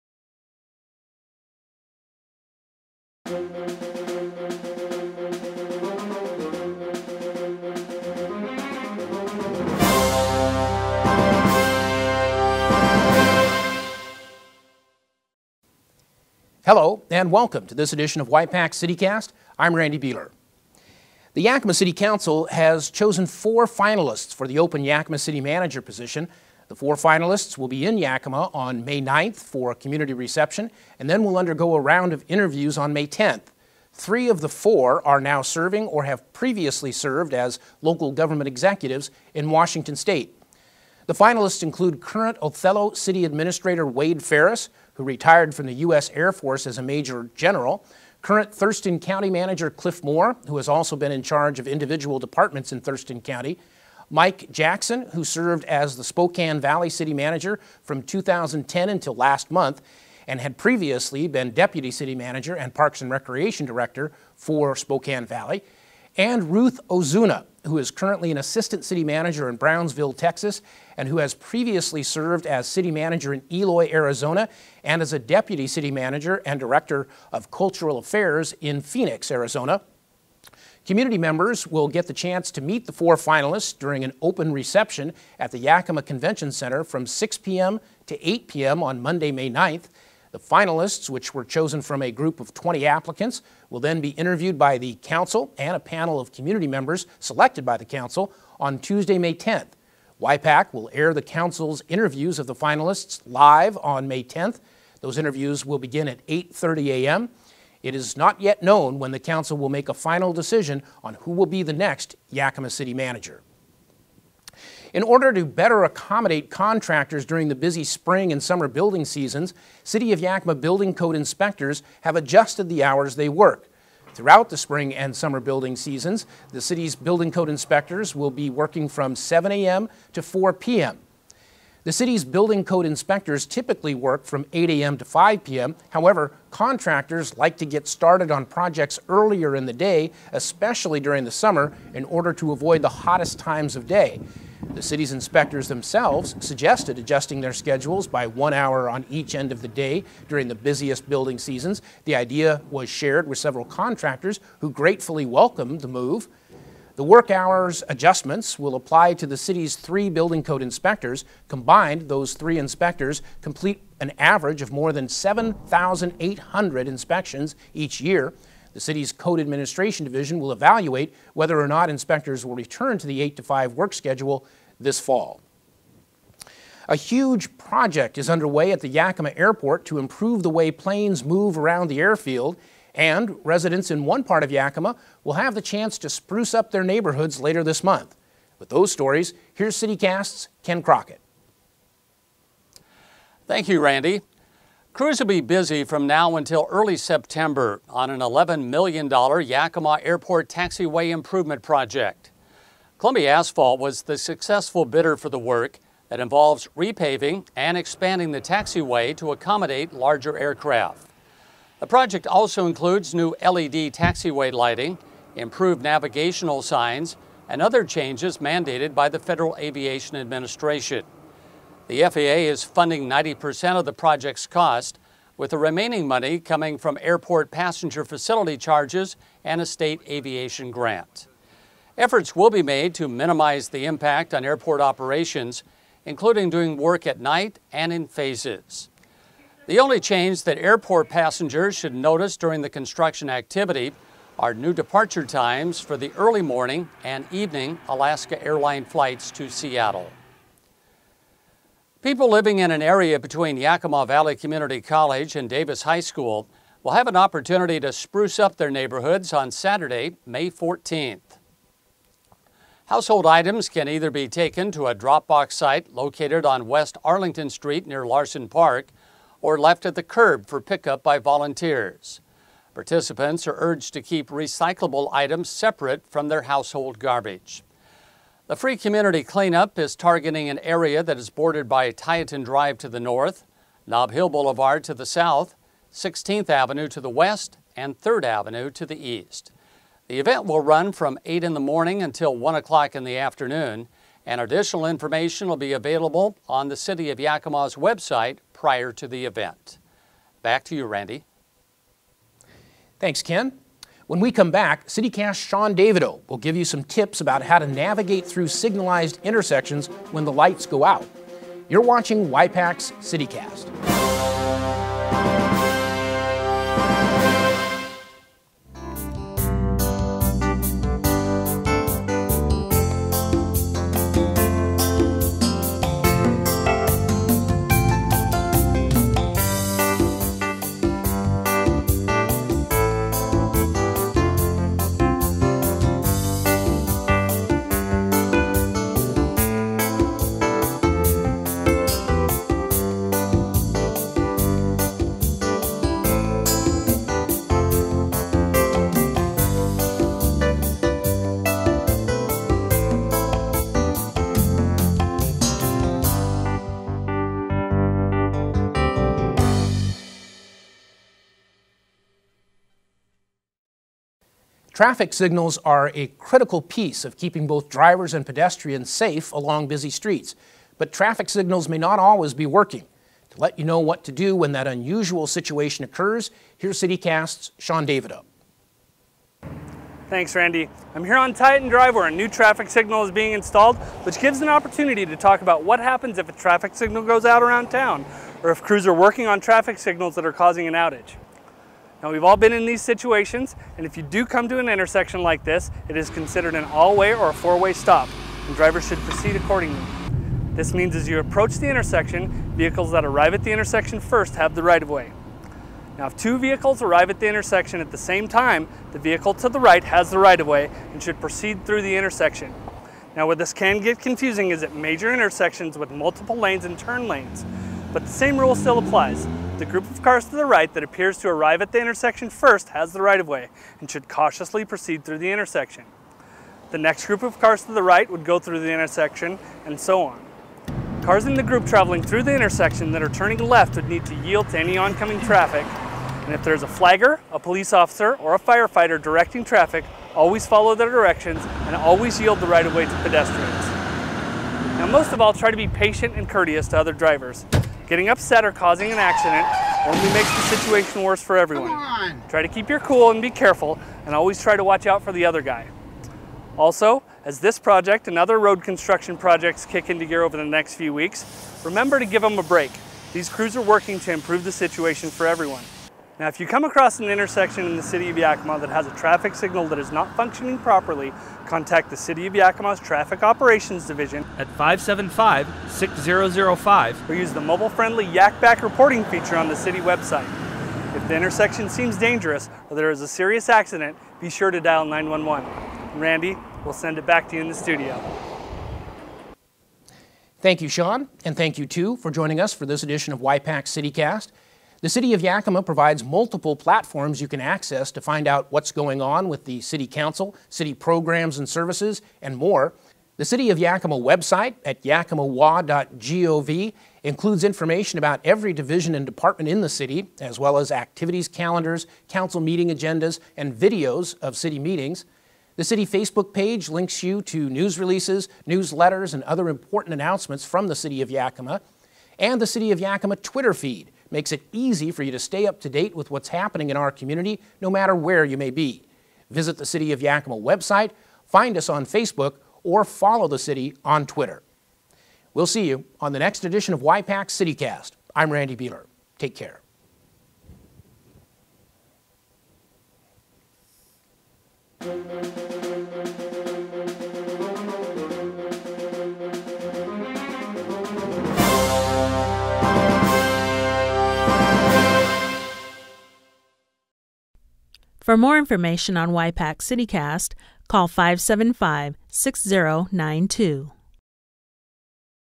Y-PAC's CityCast - 46th Edition
Watch the latest edition of Y-PAC's CityCast for accurate news and information from the City of Yakima. The 46th edition of CityCast contains reports on a major construction project underway at the Yakima airport, information on a community clean up event coming up for central Yakima residents and a report on how City of Yakima building Inspectors are helping accommodate local contractors. CityCast host Randy Beehler has information on the Yakima City Manager candidate finalists and CityCast reporter Sean Davido has tips on how to navigate an intersection if a traffic signal goes down. Find out all this and more information in the latest edition of Y-PAC's CityCast on the City of Yakima's YouTube Channel.